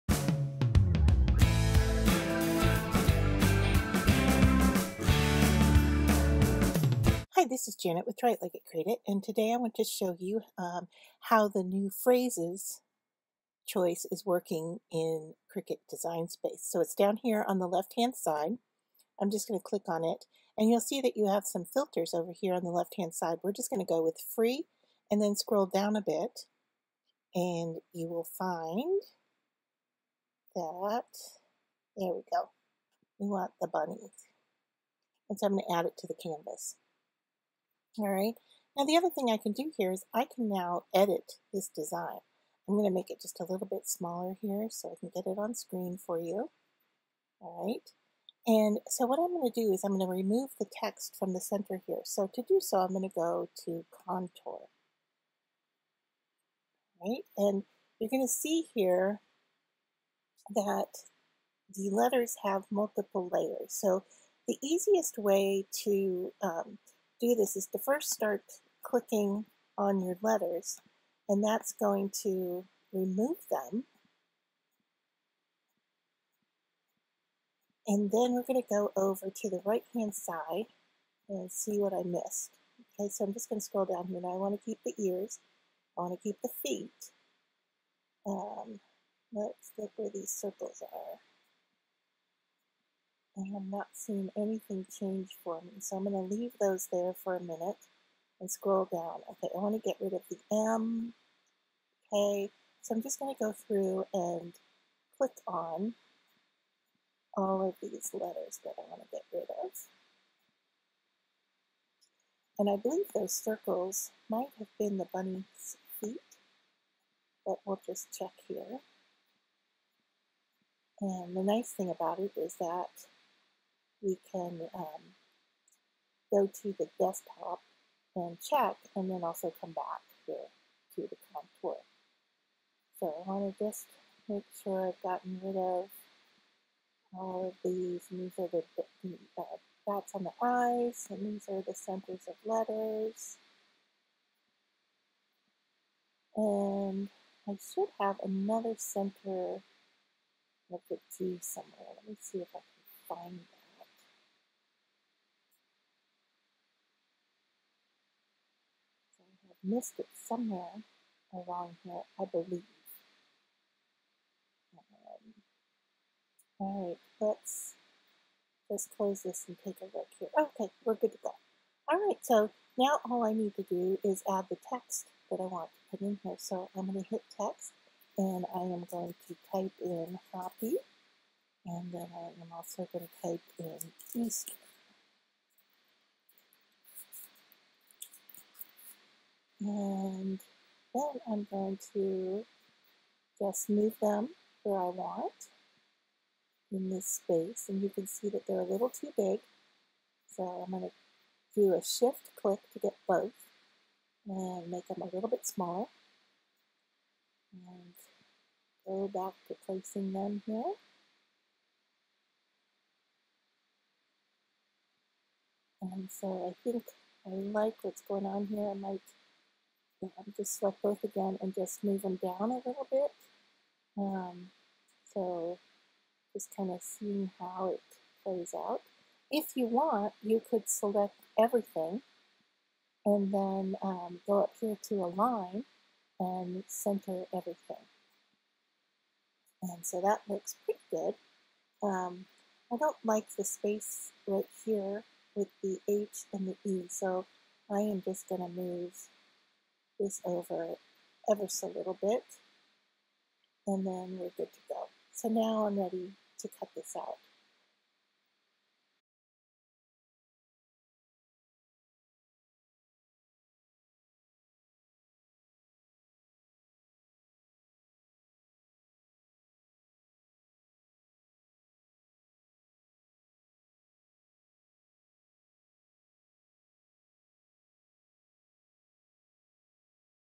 Editing The New Quotes Feature in Cricut Design Space
Learn about Cricut Design Space's new Quote feature and how you can edit them.